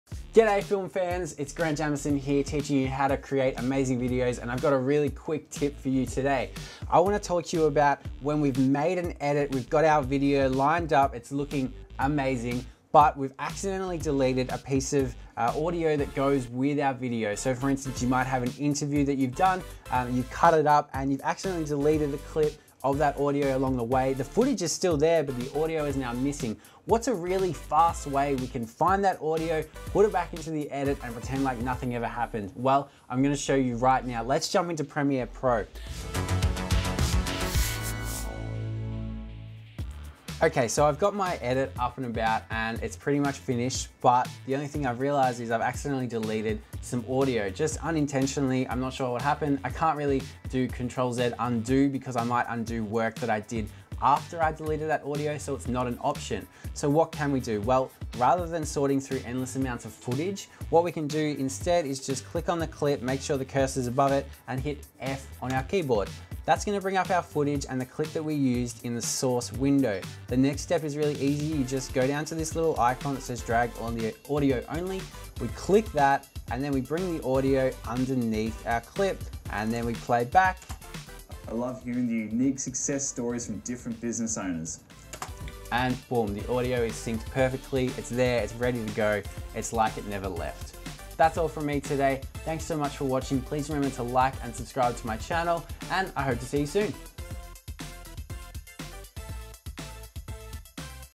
Quick Tip: Save Time Editing With Frame Match!
How to match frames in Premiere Pro - super useful if you've deleted audio in your timeline while editing, and can't undo!